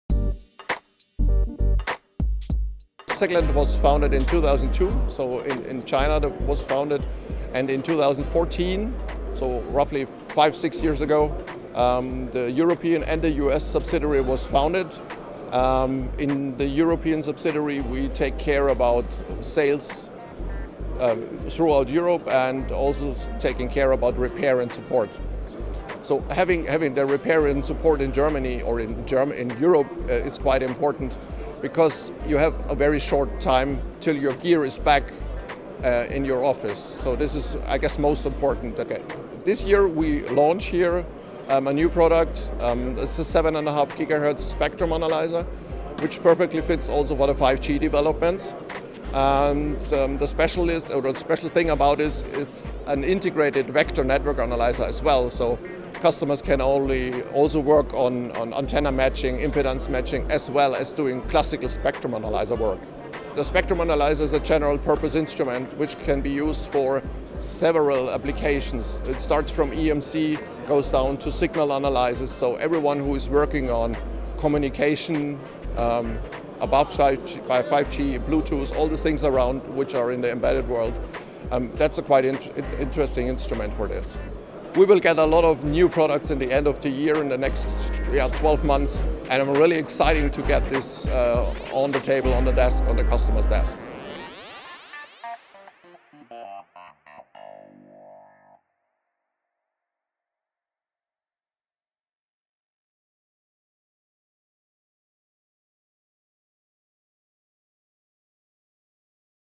Siglent's new 7.5GHz spectrum and vector network analyzer - Embedded World 2020 | Electronics Weekly
At Embedded World 2020, we caught up with Thomas Rottach of Siglent Technologies, as part of our promotional coverage for the event.

He discusses Siglent's new SVA1075X 7.5GHz spectrum and vector network analyzer - enabling users to work on antenna matching and impedance matching, as well as doing classic spectrum analysis work, for example, suitable for everyone working around communications.

Thank you to Thomas for his time.

Video: David Berman, Ash Marshall, Emap.